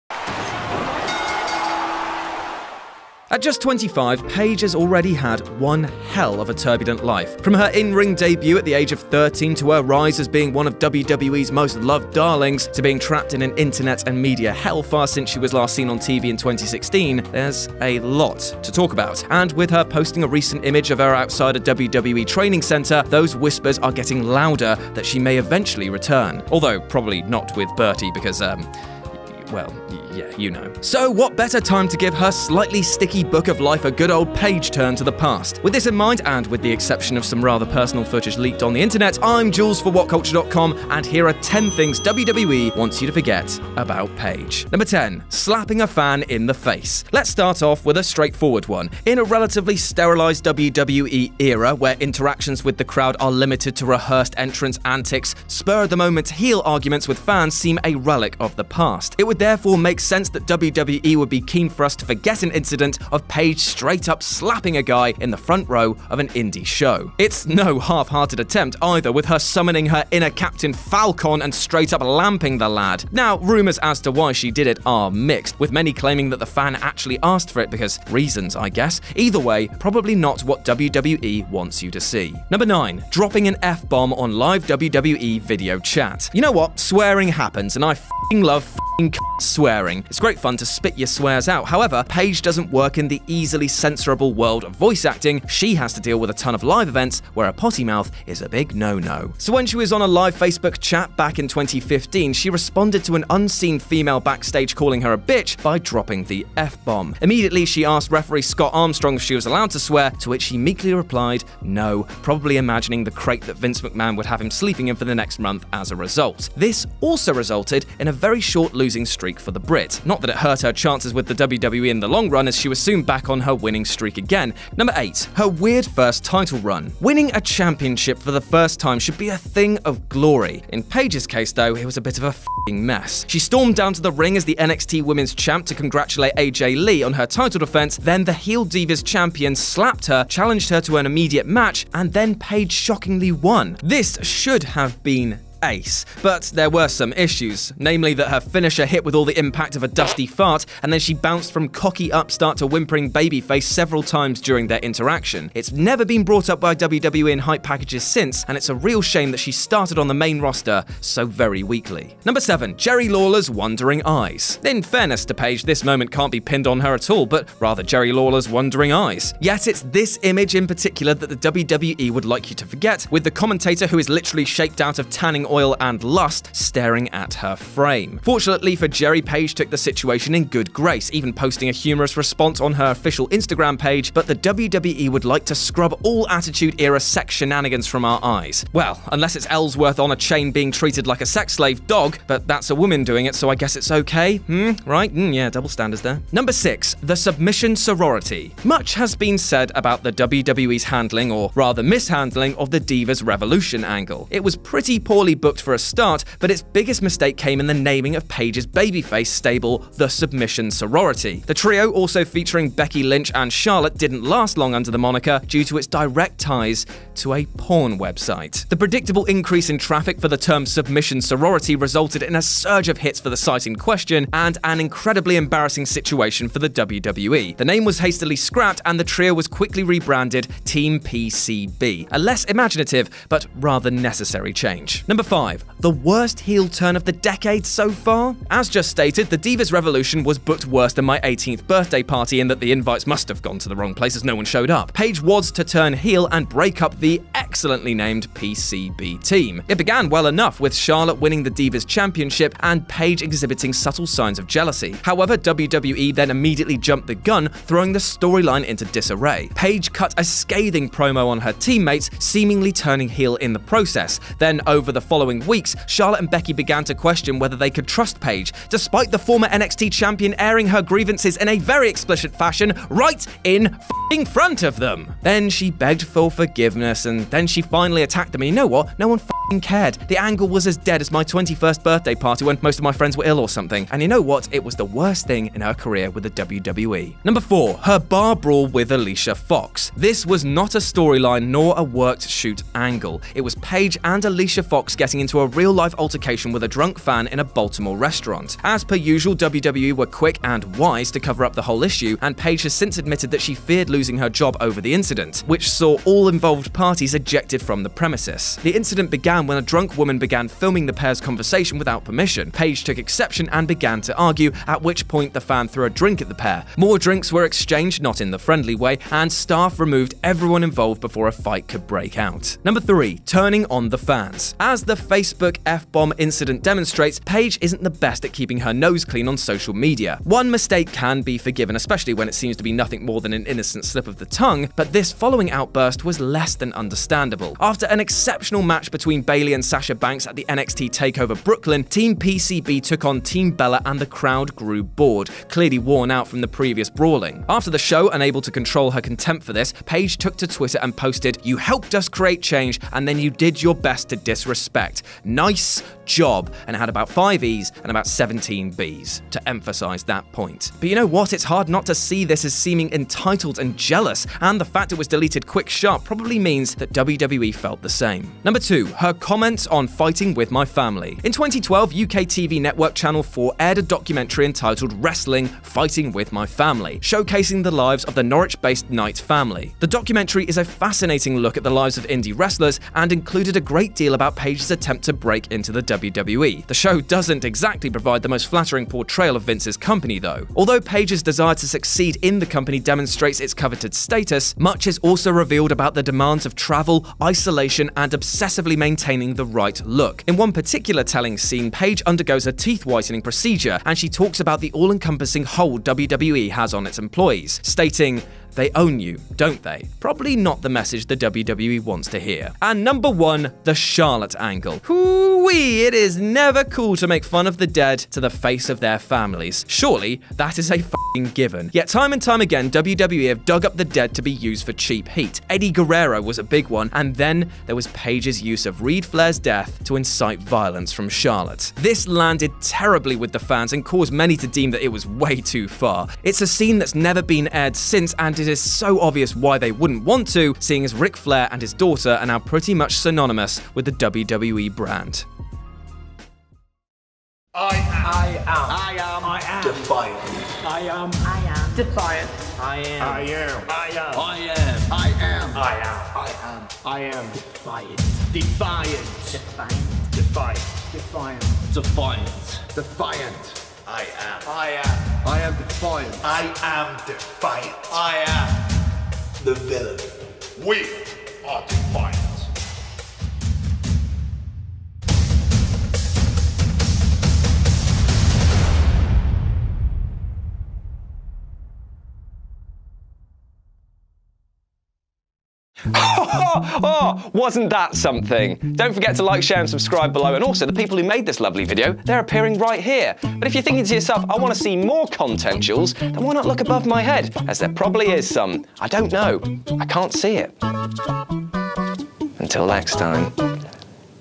10 Things WWE Wants You To Forget About Paige
One of WWE's most controversial women, in and out of the ring...